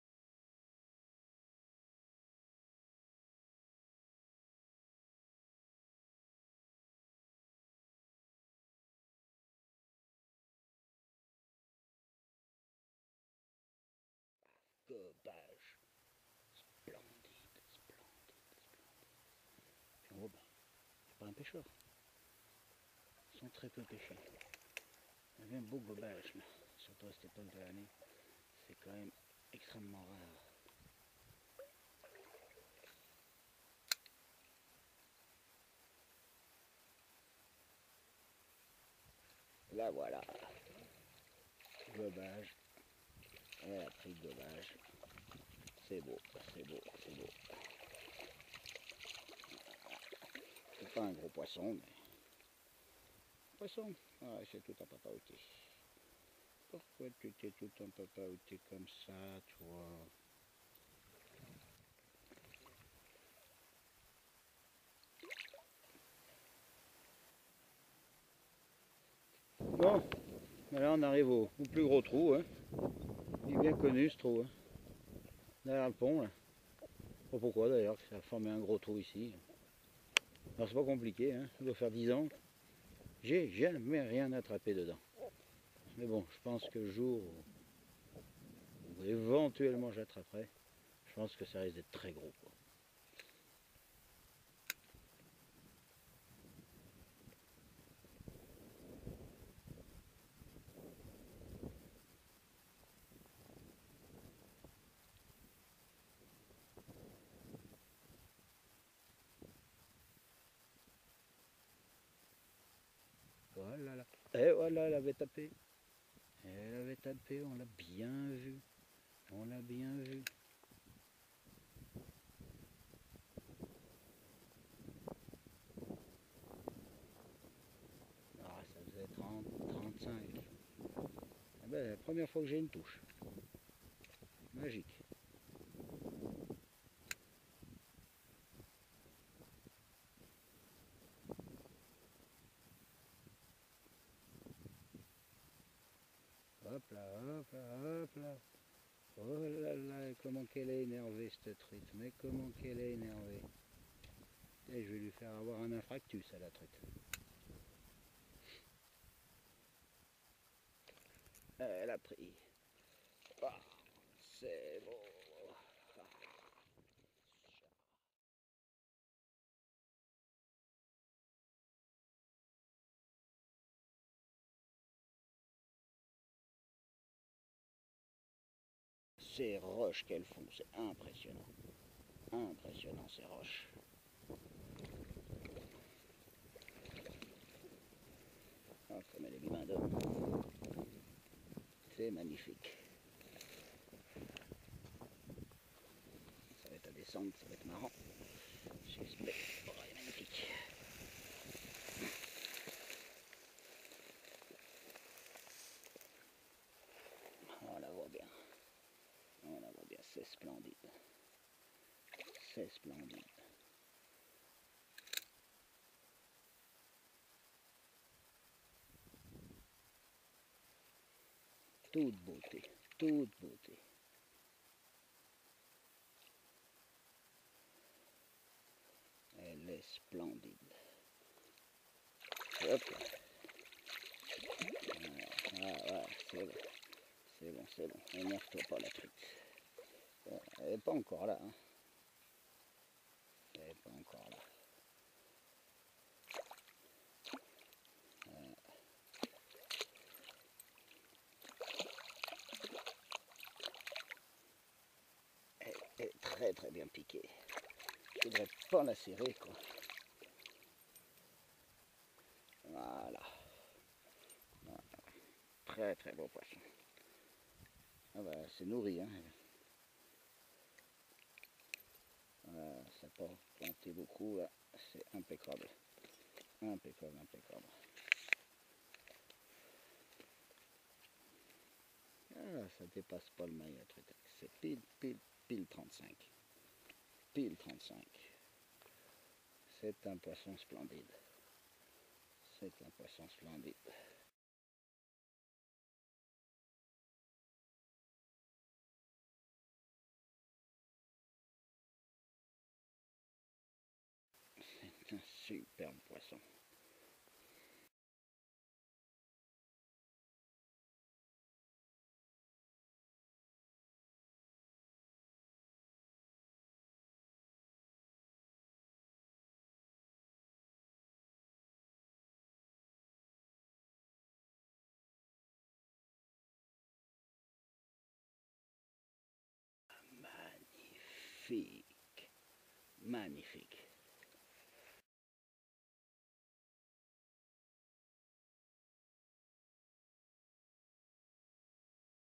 Une fario de plus d'une livre, en No Kill, dans le Gouffre de l'Houle
Jura - Une fario de plus d'une livre, en No Kill, dans le Gouffre de l'Houle, comme si vous y étiez, avec
Des aventures de pêche en full HD ...